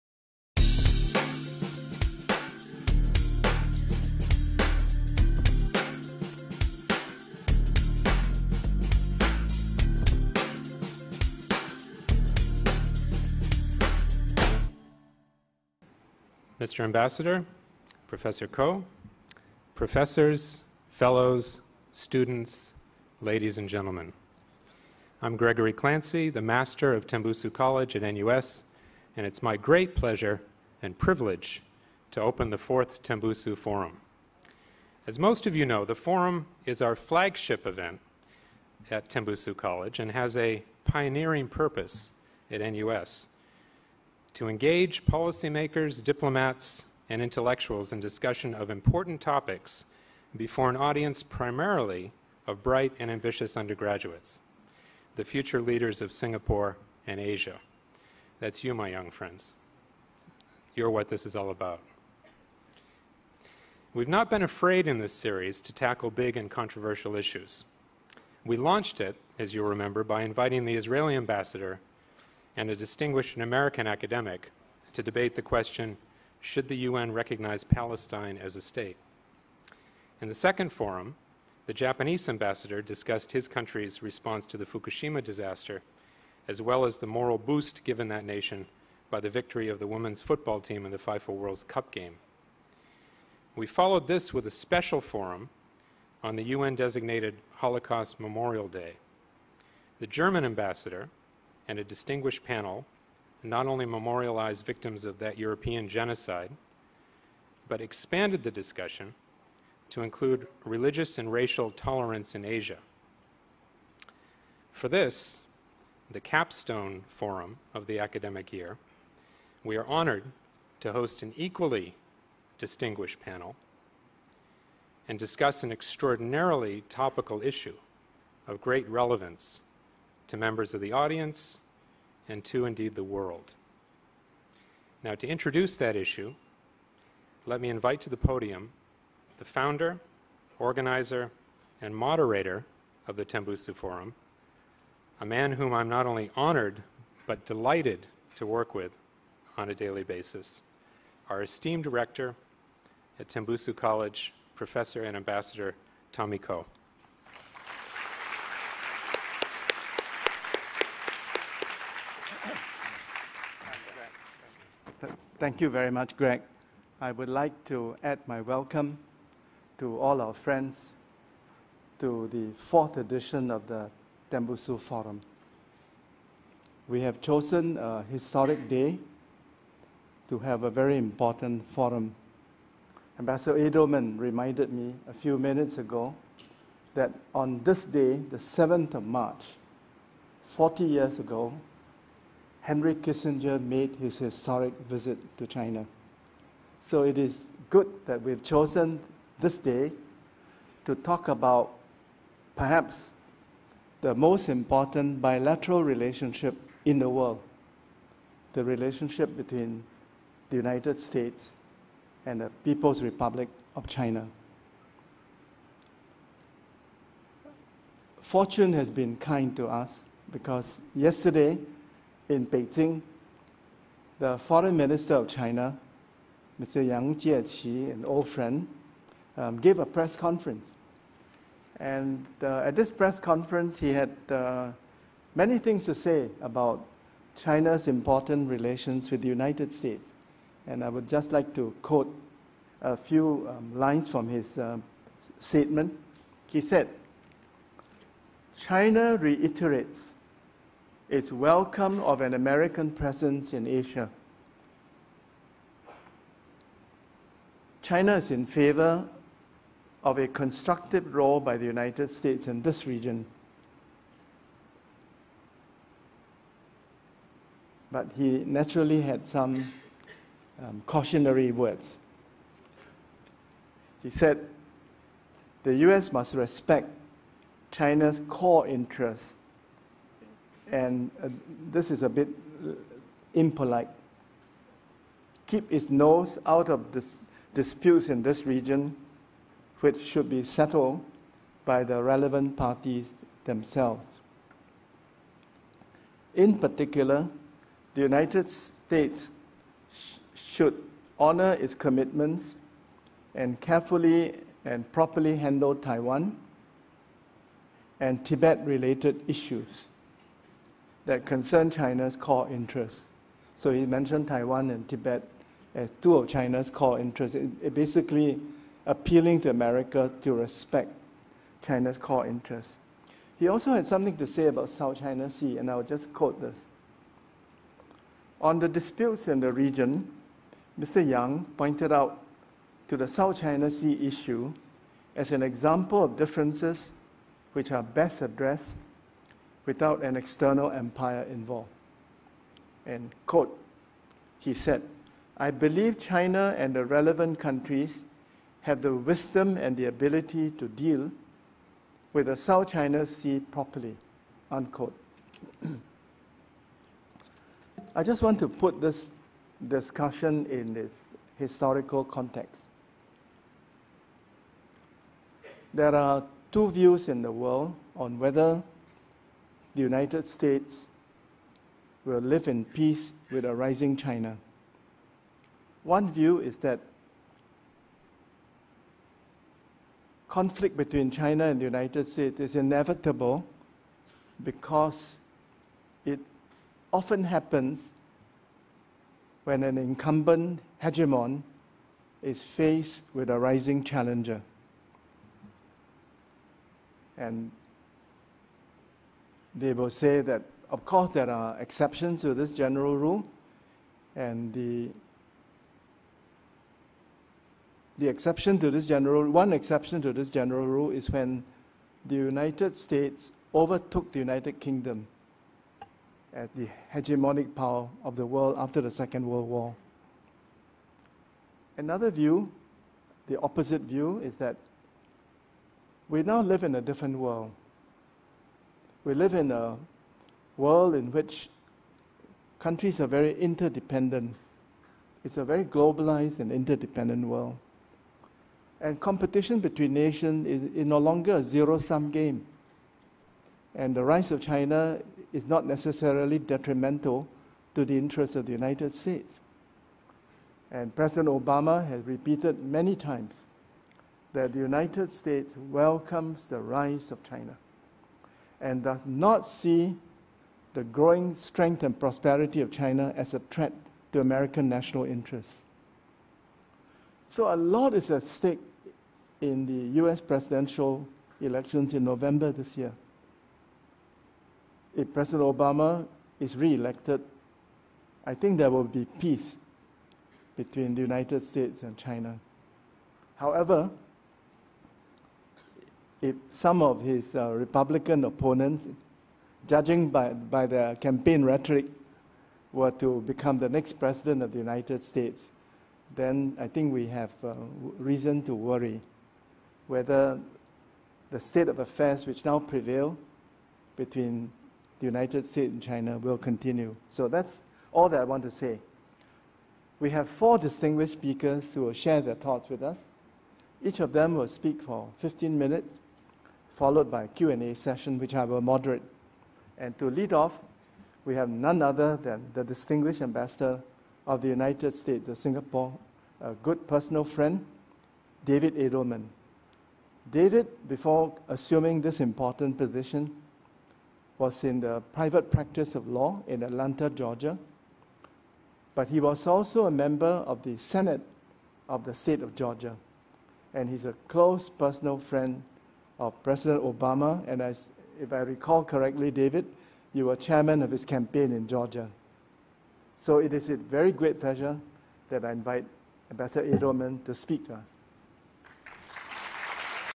Tembusu Forum: Will the U.S. Live in Peace with a Rising China 01
Topic: Will the US Live in Peace with a Rising China?
Wednesday, 7th March 2012
Tembusu Multi-purpose Hall

Moderator:
Professor Tommy Koh, Rector, Tembusu College

Speakers:
His Excellency David Adelman
United States Ambassador to Singapore
U.S. Embassy in Singapore

Prof. Zheng Yong Nian
East Asian Institute, NUS

Prof. Terry Nardin
Head, Department of Political Science, NUS

Dr. Huang Jing
Lee Kuan Yew School of Public Policy, NUS